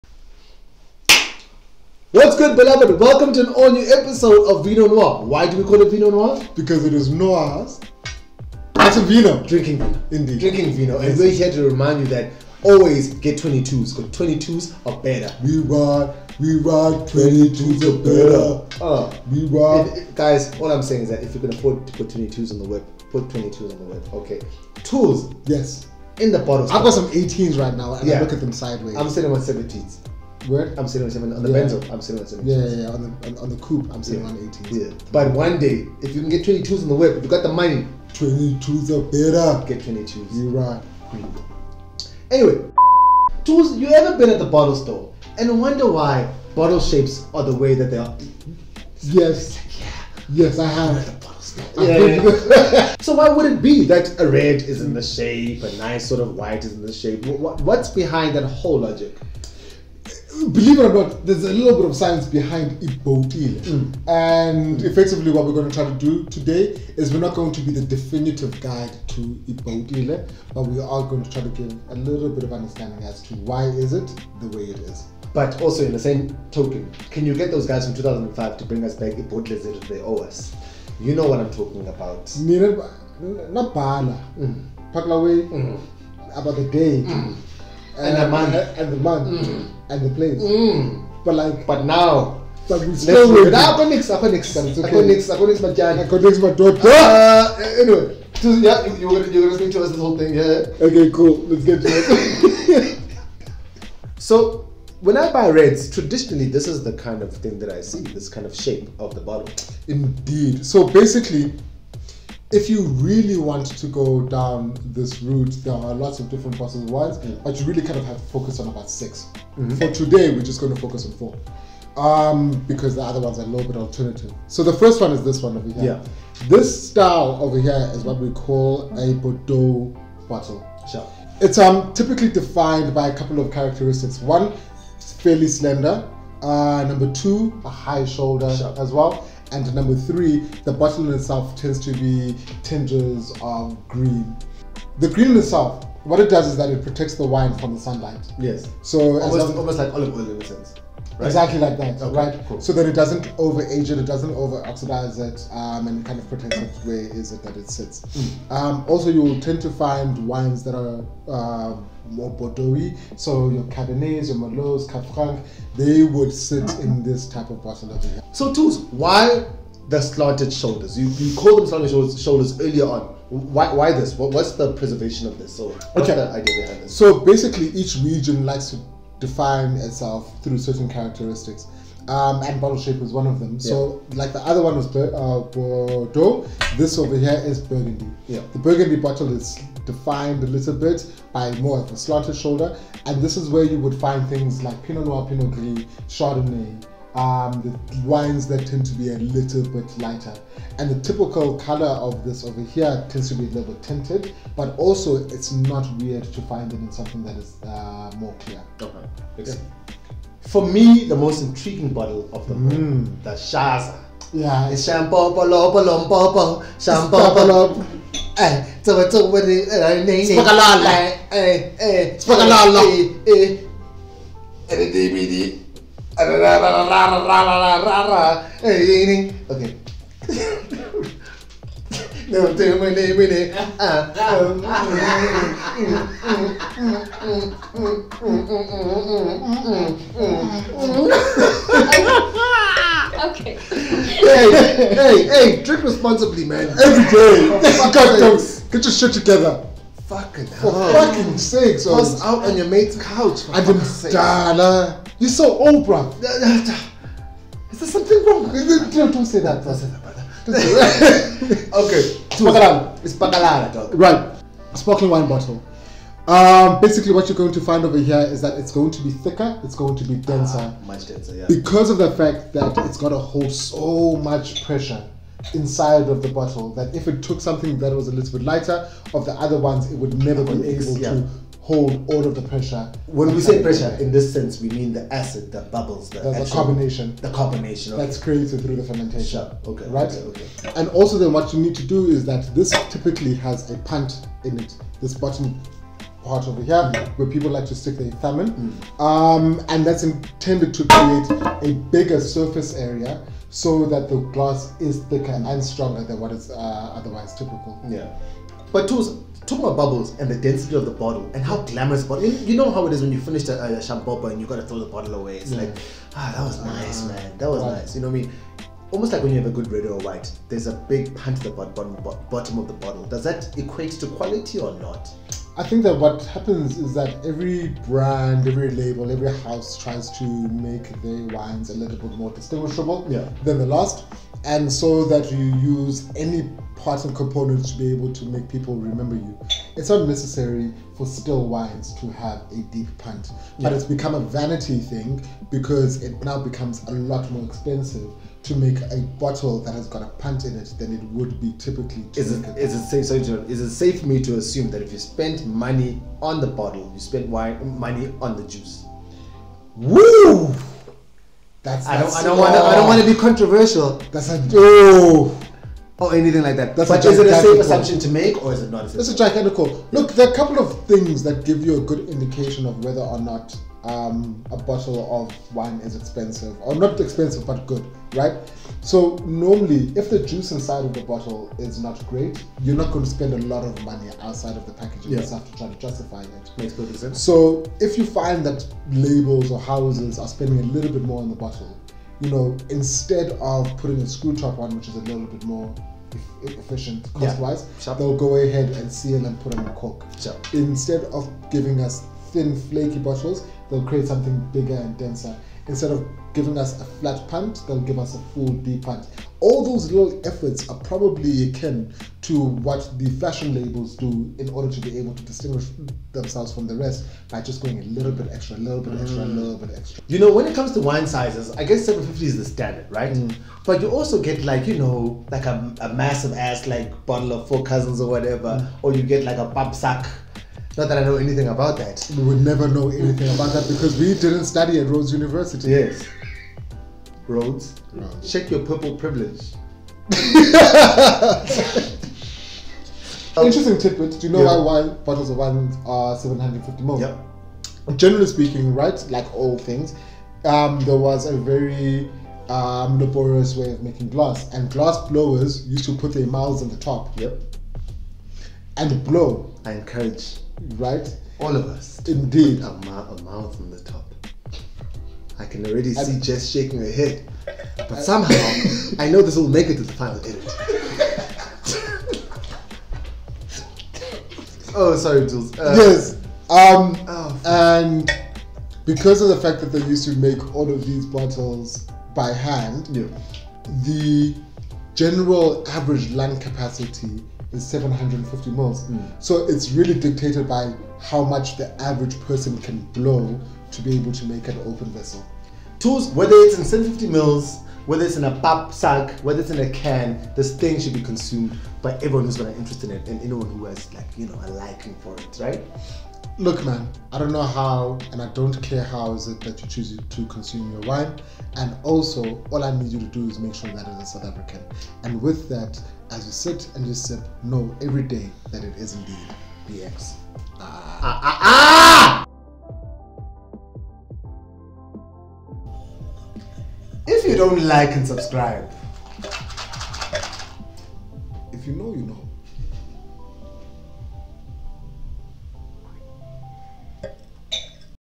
BOTTLES | 3 wine bottles and 22-inch rims
What's good beloved!

Today we're looking at the main bottle shapes and everything around it. We're discussing the bordeaux bottle, the burgundy bottle, and the classic champagne bottle.